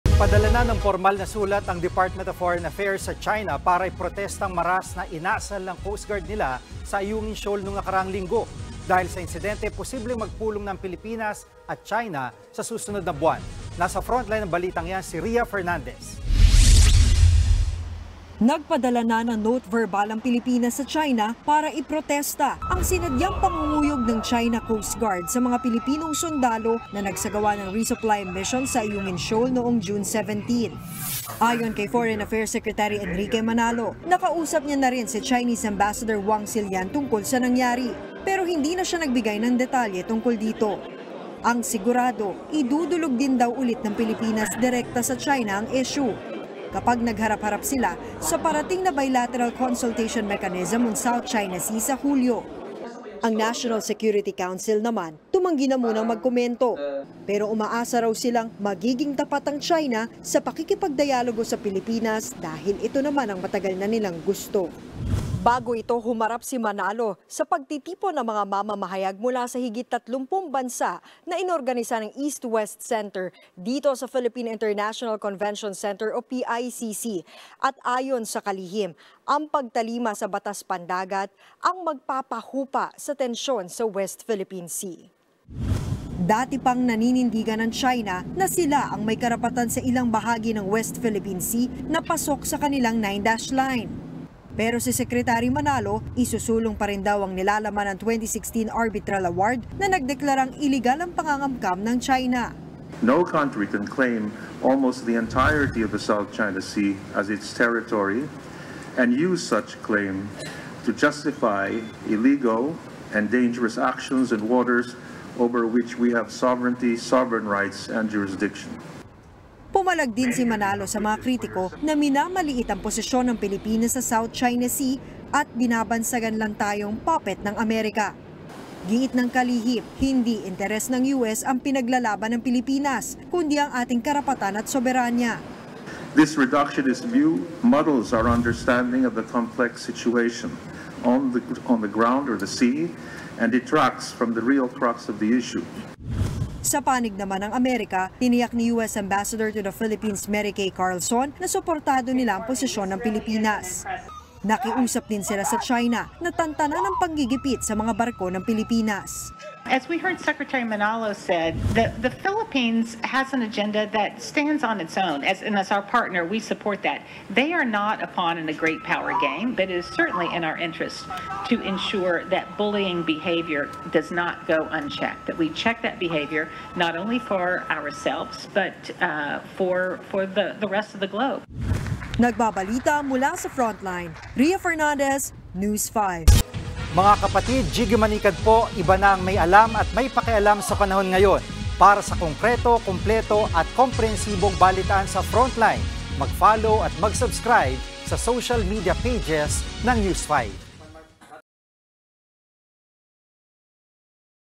Pilipinas, nagpadala ng note verbale sa China kaugnay sa insidente sa Ayungin Shoal noong June 17
FrontlinePilipinas | Nagpadala na ng pormal na sulat ang Department of Foreign Affairs DFA sa China para iprotesta ang marahas na inasal ng coast guard nila sa Ayungin Shoal noong nakaraang linggo.

Dahil sa insidente, posibleng magpulong na ang Pilipinas at China sa susunod na buwan. News5 | via Ria Fernandez